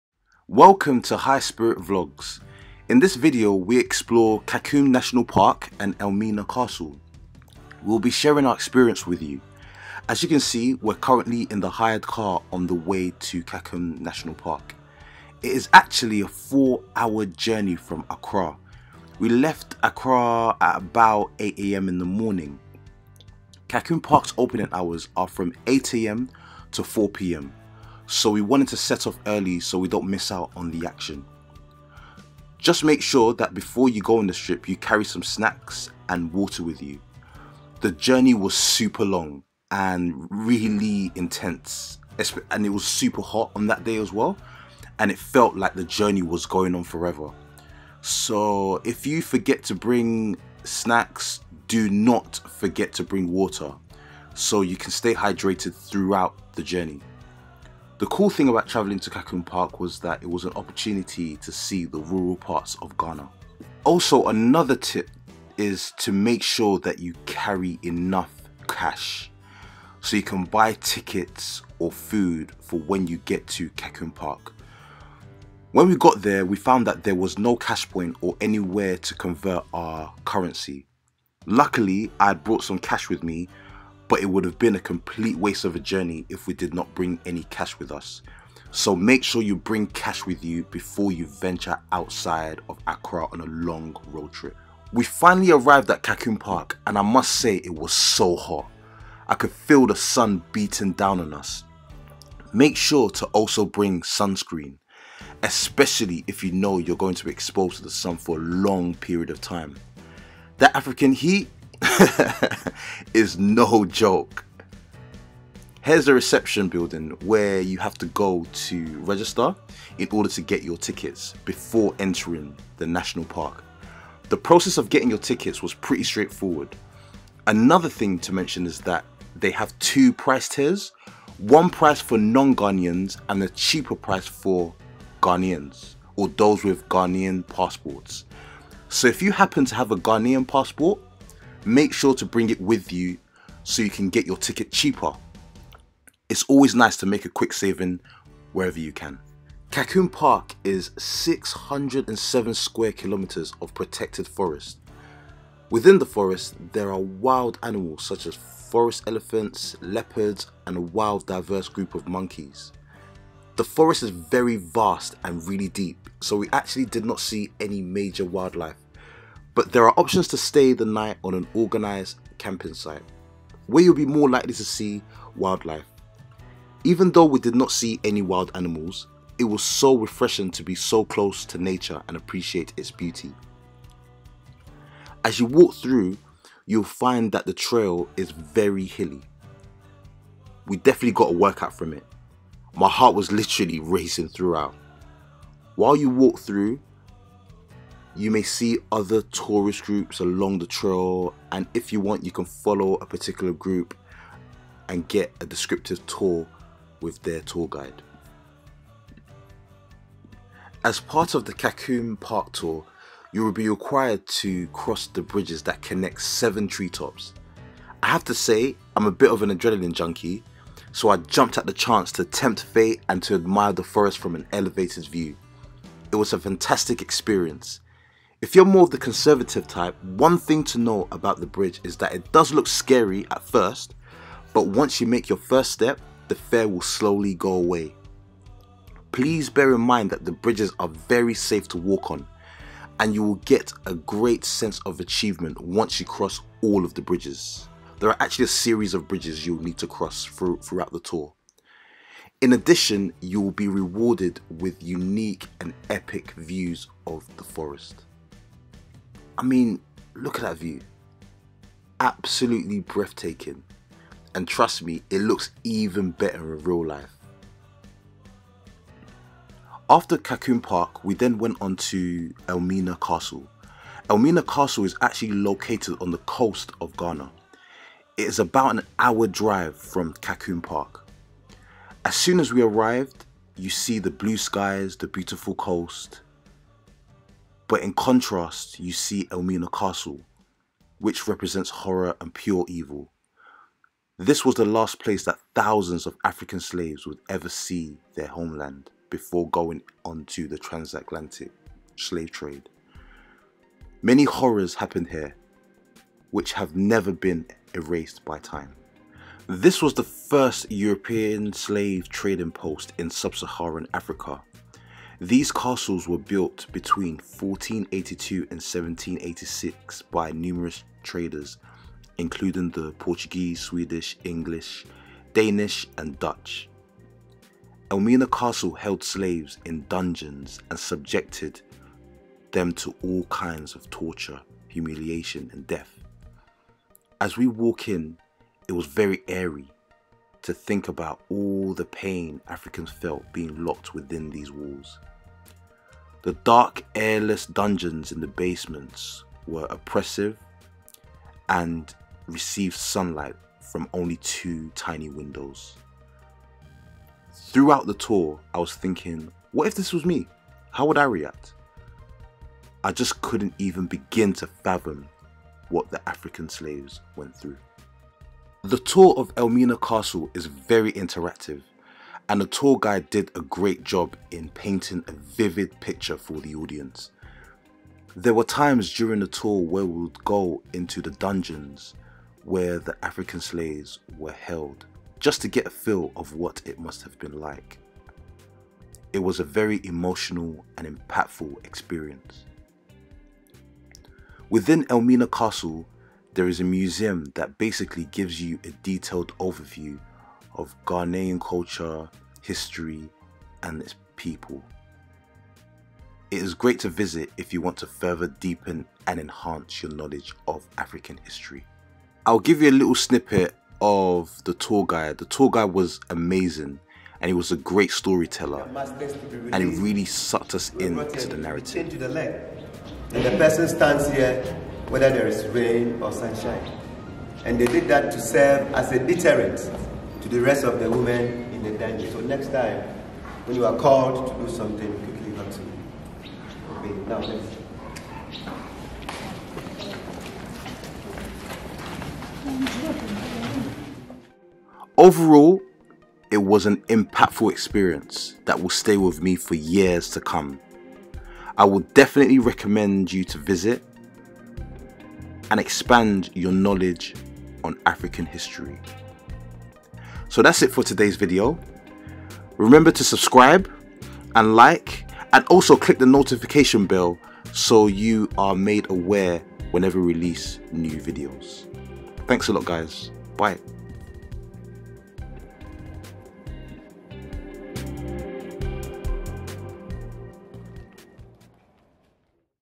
Ghana Travel Video: Kakum Park and Elmina Castle Visit - What To Expect
In this Ghana travel video vlog we travelled to explore Kakum National Park and Elmina Castle. The visits enabled us to see more of the natural environment in Ghana and it also encouraged us to be more educated on African History. The trip to Elmina Castle was a impactful and eye-opening experience. I would recommend it to everyone to visit. In this video we will do a walkthrough of our visits and we will share with you our experience visiting both places and share some tips. Enjoy!

Canon Camera: